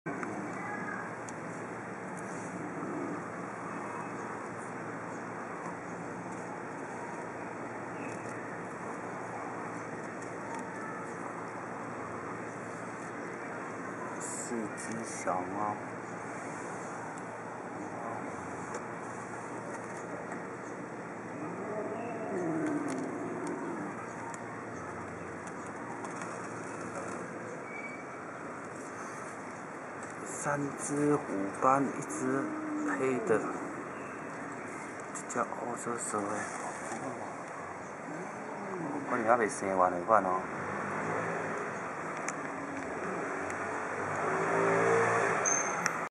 TNR 變做月內，母貓在捕貓籠內生了四隻小貓 20110710 18:26
TNR 變做月內，母貓在捕貓籠內生了四隻小貓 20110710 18:26